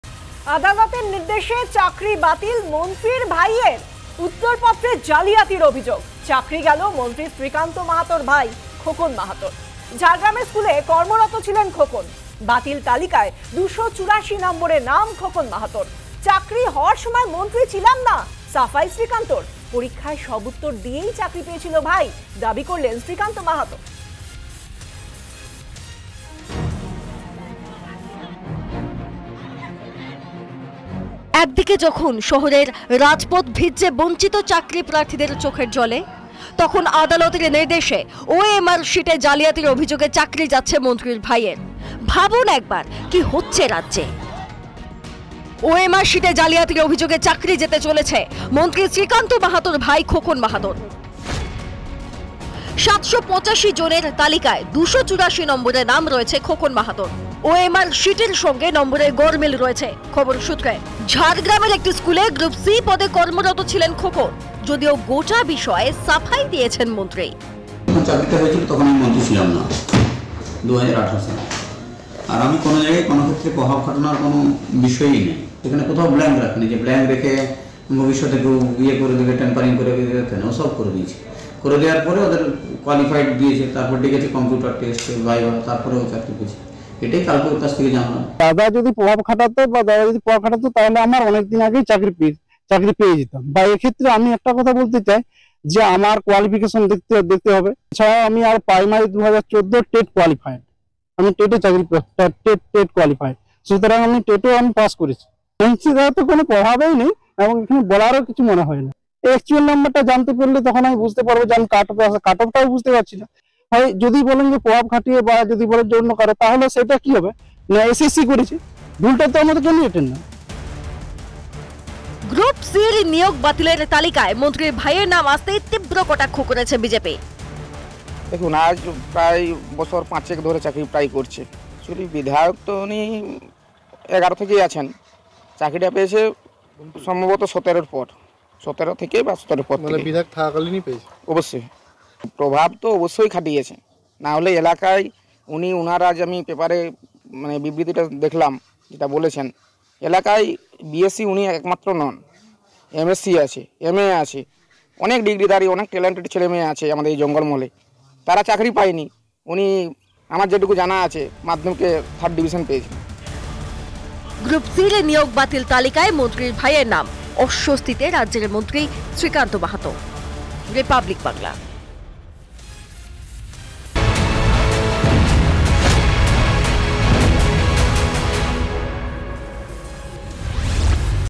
ভাইয়ের চাকরি বাতিলে অস্বস্তিতে মন্ত্রী শ্রীকান্ত মাহাত। কী সাফাই দিলেন তিনি? দেখুন ভিডিয়ো
Srikanto Mahato | আদালতের নির্দেশে চাকরি গেল মন্ত্রী শ্রীকান্ত মাহাত-র ভাইয়ের। ঝাড়গ্রামের স্কুলে গ্রুপ সি পদে চাকরি করতেন খোকন মাহাত। ভাইয়ের চাকরি বাতিলে অস্বস্তিতে শ্রীকান্ত মাহাত। কী সাফাই দিলেন তিনি? দেখুন ভিডিয়ো

রিপাবলিক স্বাধীন।
রিপাবলিক নির্ভীক।
আপনিই রিপাবলিক।